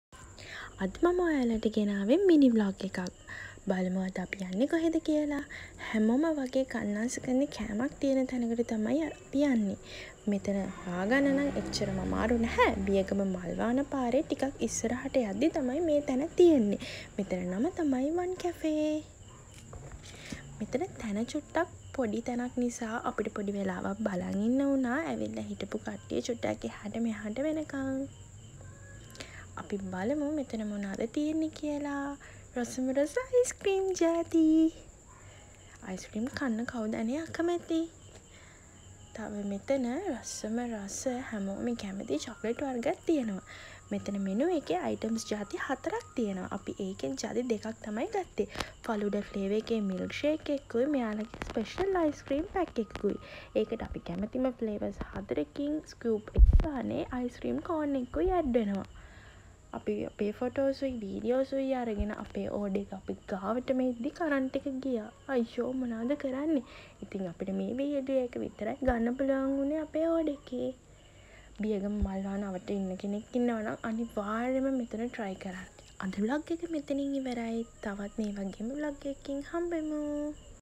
Vlog 02 | අයිස්ක්‍රීම් පෙරේතයන් සඳහාමයි | one cafe
අයිස්ක්‍රීම් වලට ආස ඔයාලා වෙනුවෙන්මයි.

මේ තැන තියෙන්නෙ බියගම මල්වාන පාරේ ටිකක් ඉස්සරහාට යද්දි. One Cafe තමයි මෙතන නම.

 One cafe එකෙ facebook page එක තමයි මේ..

Travel Treat අපේ facebook page එක

Travel Treat අපේ tik tok එක